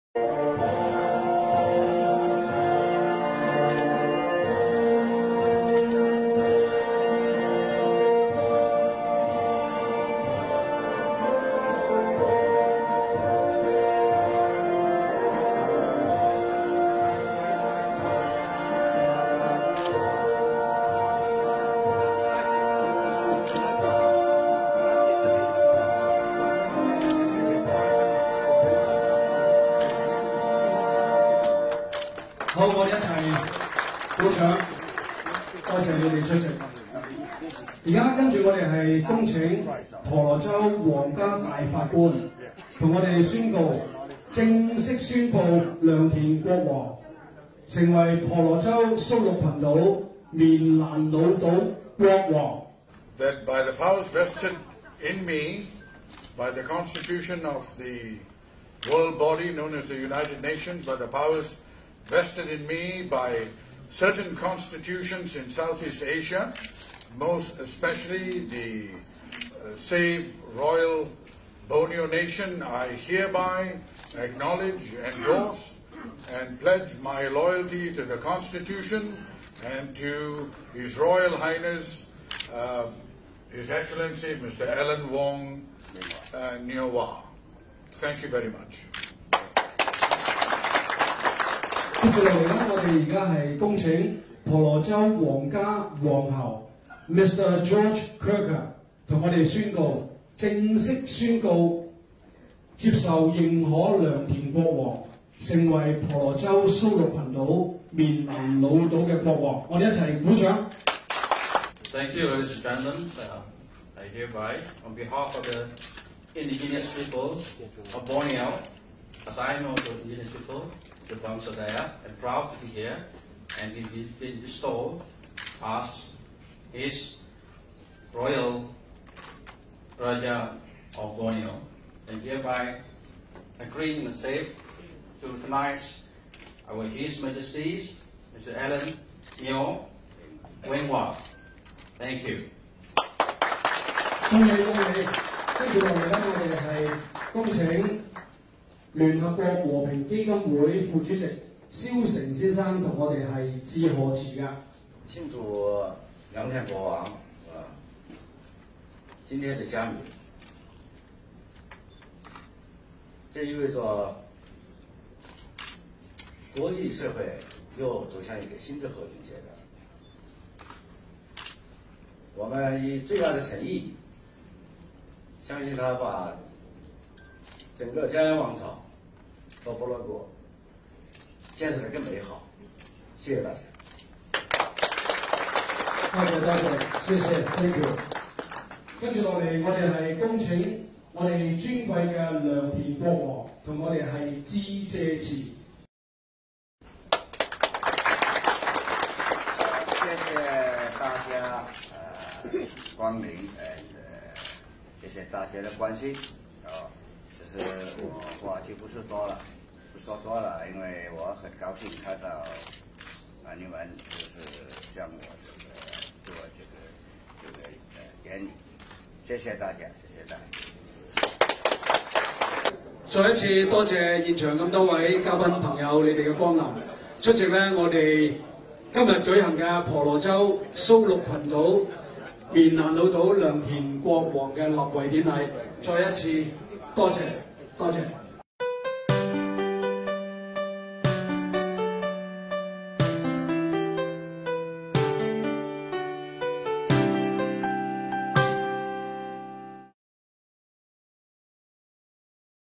Tular: Orang Cina mengaku Raja bagi Borneo, Sulu & Mindanao
Tidak diketahui pada tahun bila video ini dirakamkan, namun video ini telah tular pada hari ini (28/10/2020) di dalam aplikasi Whatsapp.